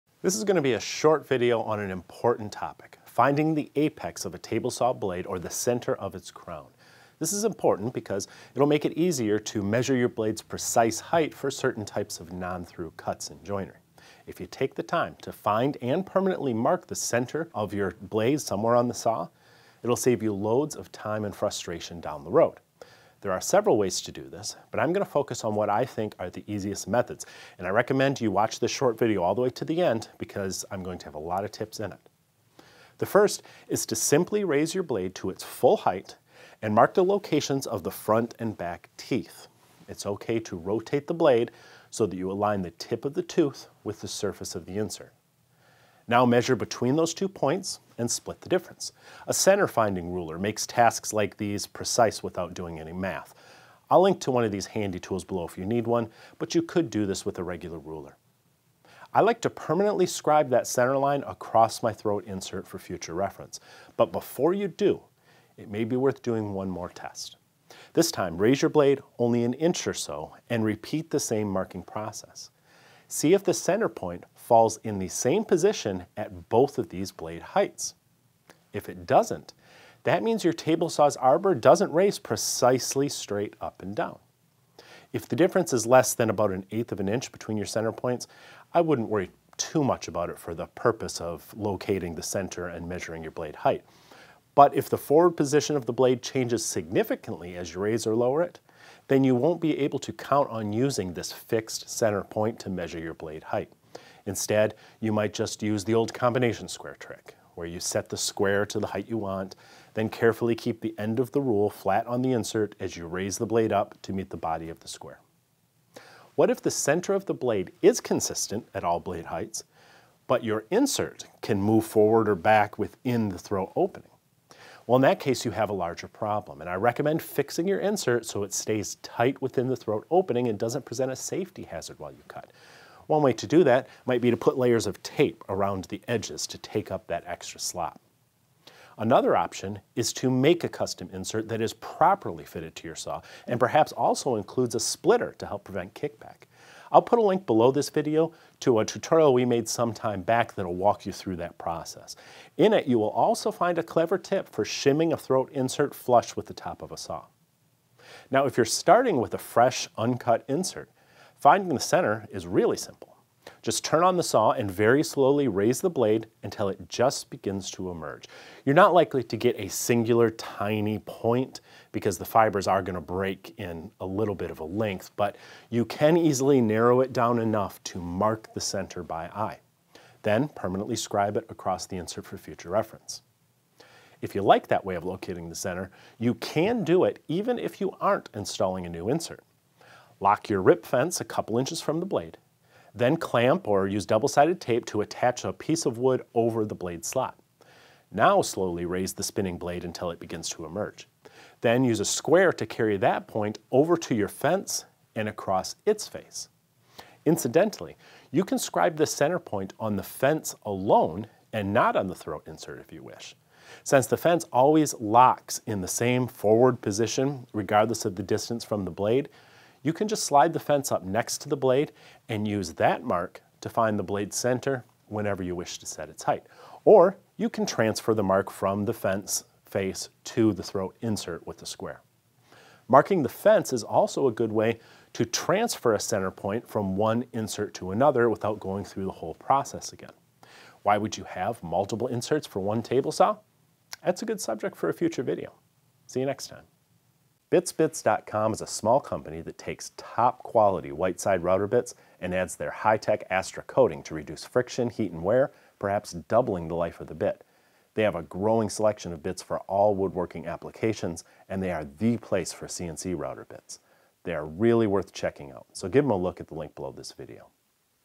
Why you should mark the center of your saw blades
The fastest way to find the apex of a table saw blade- and why it matters!
▼EXPAND THIS SECTION FOR MORE RESOURCES RELATED TO THIS VIDEO▼